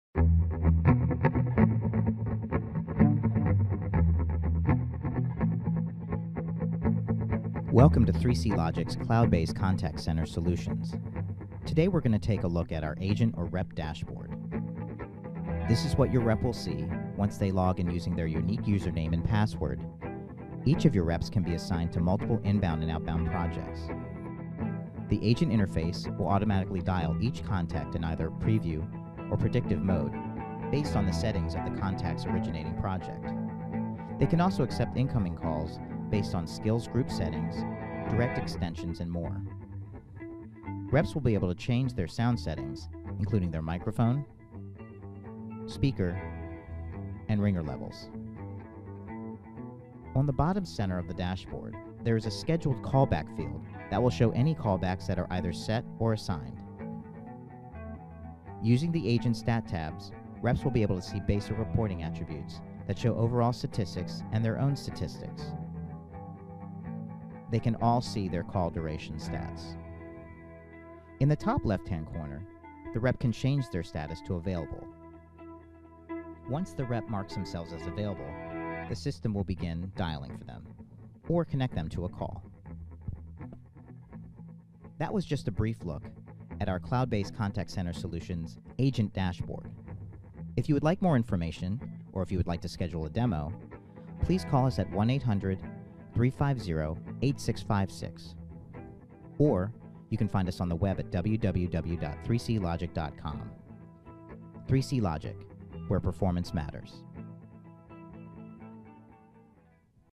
3CLogic Cloud Contact Center Software, Rep or Agent Dashboard Software Demo
This video shows a high level glimpse of 3CLogic's cloud contact center software rep/agent dashboard.